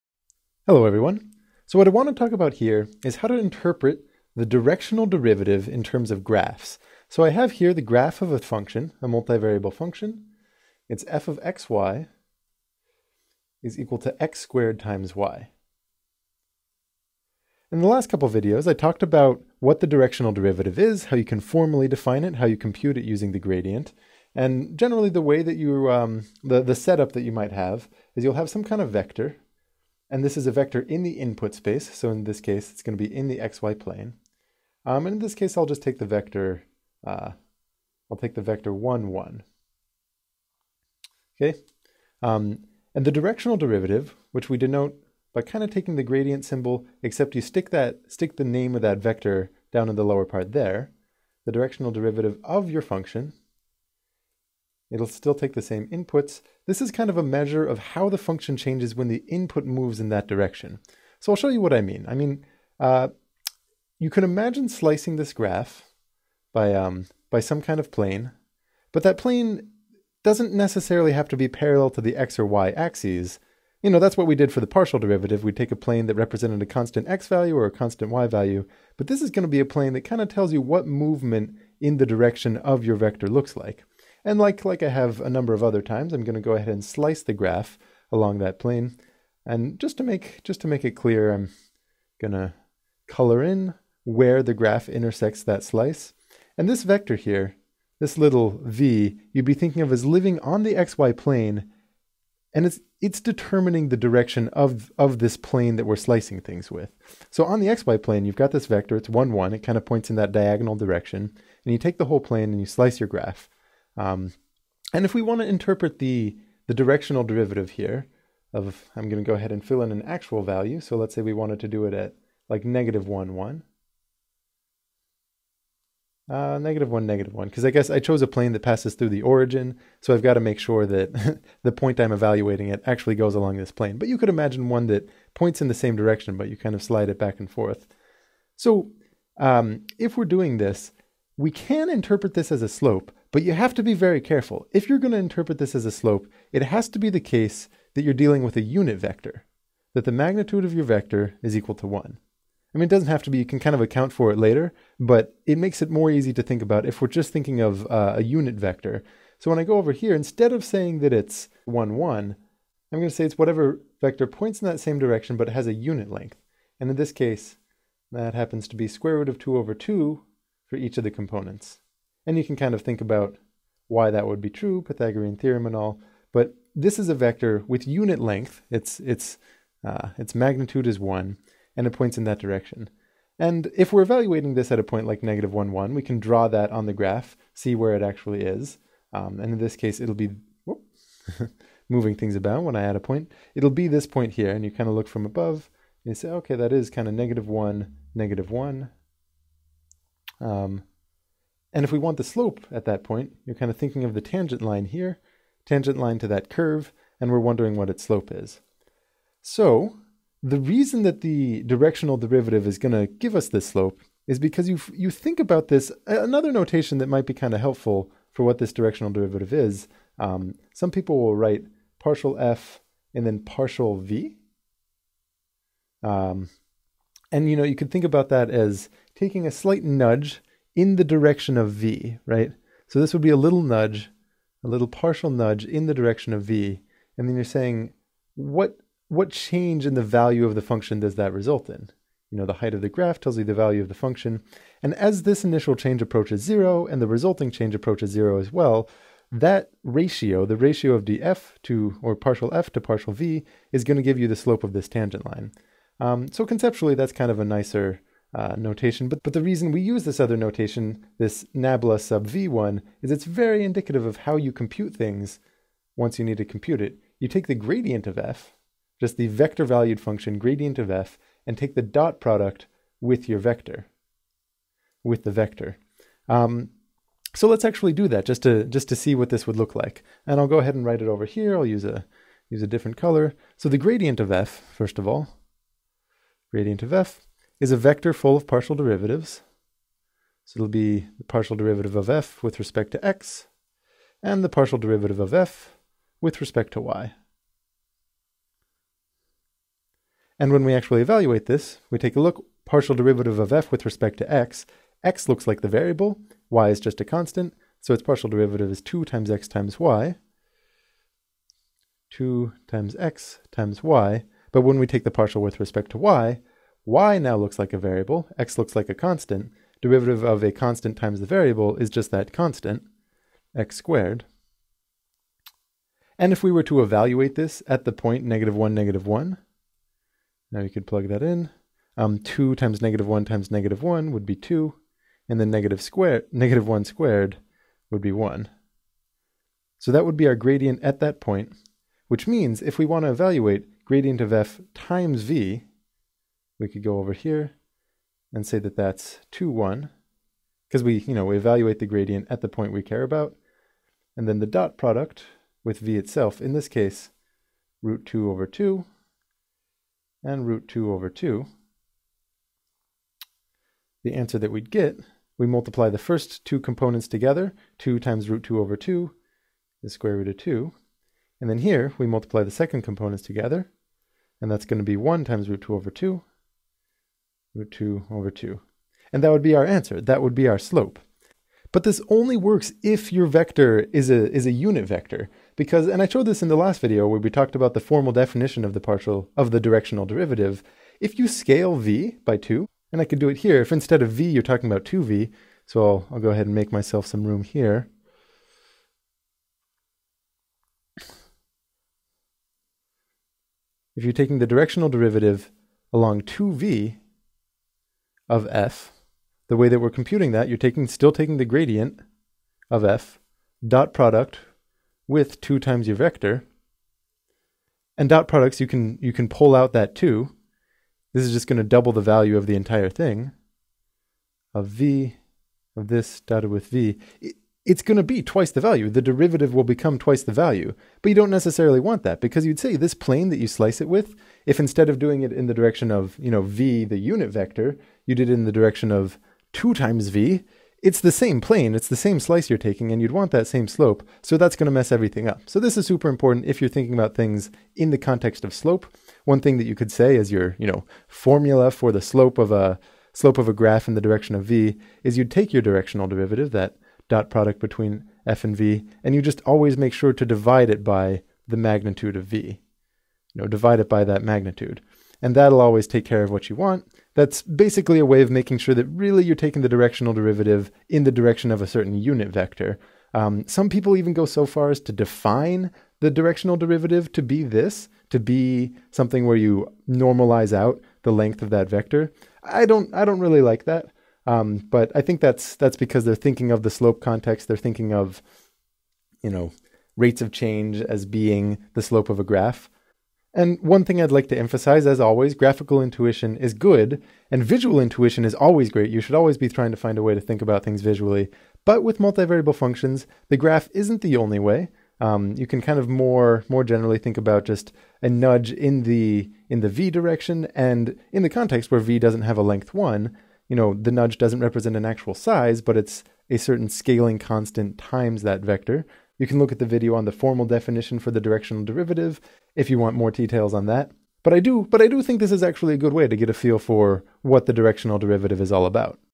Directional derivatives and slope
Courses on Khan Academy are always 100% free. Start practicing—and saving your progress—now

The directional derivative can be used to compute the slope of a slice of a graph, but you must be careful to use a unit vector.